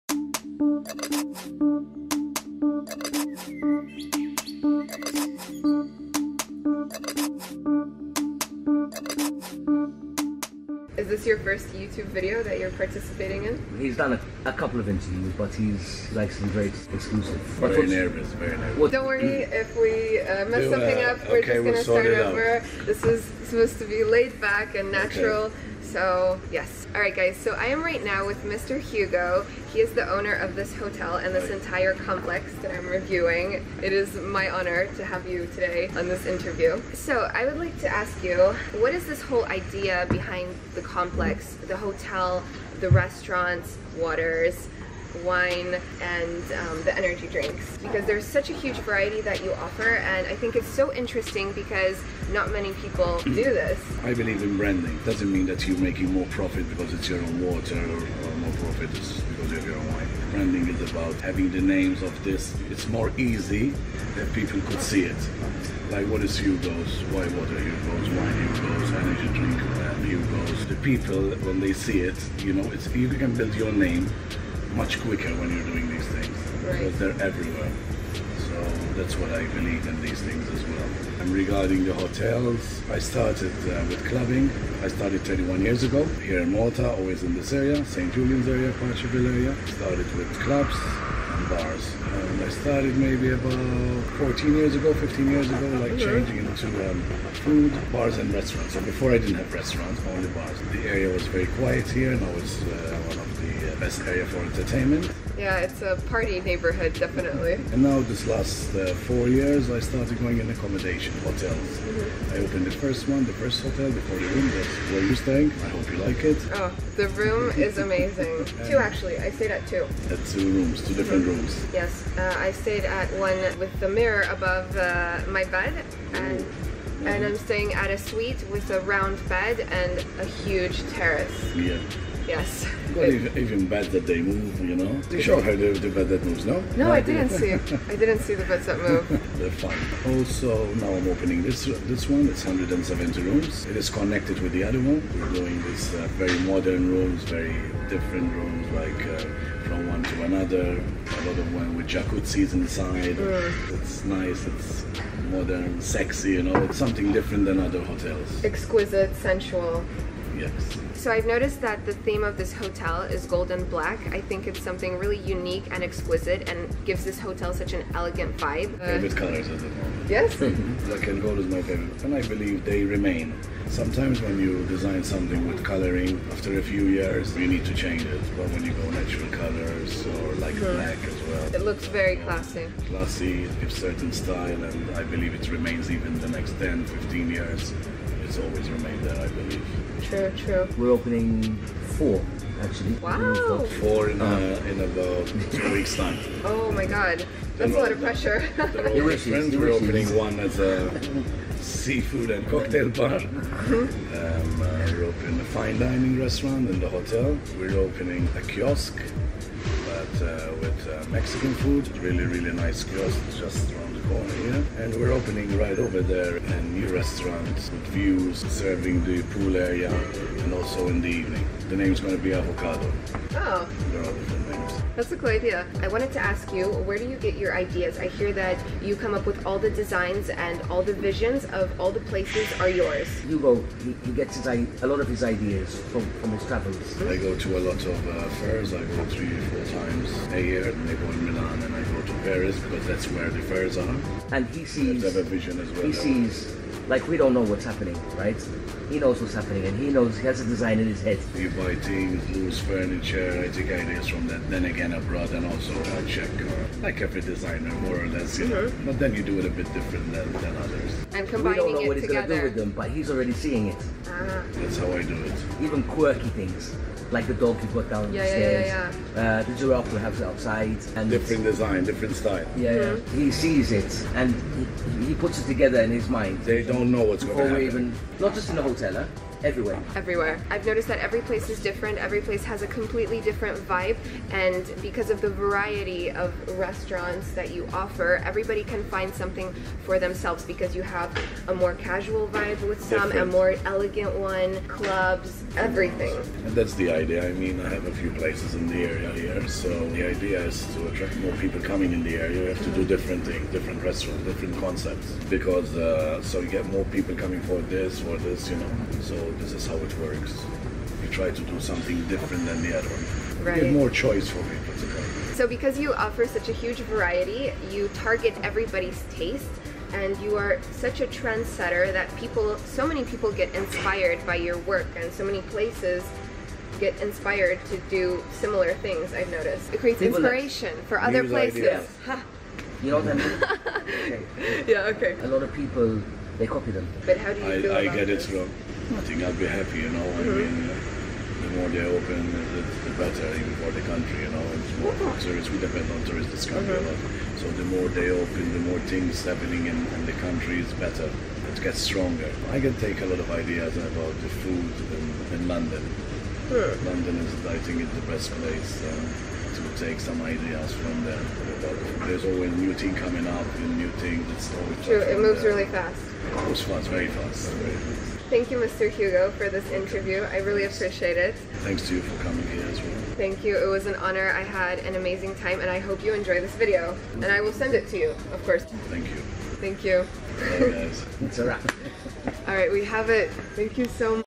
HUGO CHETCUTI - The Last Interview feat. Aria Martelle
HERE IT IS Hugo Chetcuti Last Interview.

As many of you already know, 1 year ago I moved to Malta because i fell in love with the island and it made me feel 100% at home.
While living here and getting to know more about Malta one of the things that really impressed me was the entire Hugo's complex (Hotel, Bars, Restaurants), i found it so unique & inspirational for one person to have so much to offer as a brand and care about each and every detail.
2 weeks ago I had the opportunity to review Hugo's Boutique Hotel,the entire Complex of places, aswell as meet Mr. Hugo himself.
I was pretty nervous because Mr.Hugo his work & creativity was something I was looking up to for a very long time and it was an honor to me to be able to meet him in person.
He was extremely kind and agreed for me to interview him for my channel (It was his first interview with a YouTuber).
We talked about what ispired him, the idea behind the brand, the beginnings of growing his empire & his legacy.
Our conversation on & off camera, was honest, inspirational and gave a lot to think about not only business wise, but also about life, creativity and people you surround yourself with.
I am so upset and in shock about what happened, because not long ago I told Mr. Hugo that I will send him the video of our interview and he was so excited that he would see it.
Sadly, I was one of the last people to interview him... if not the last person.
Now all I can do is make the best out of the memorable information he shared, so his legacy can live on and inspire others.
My great condolences to the Chetcuti family, and other people that are devastated by this tragic news.
RIP Hugo Chetcuti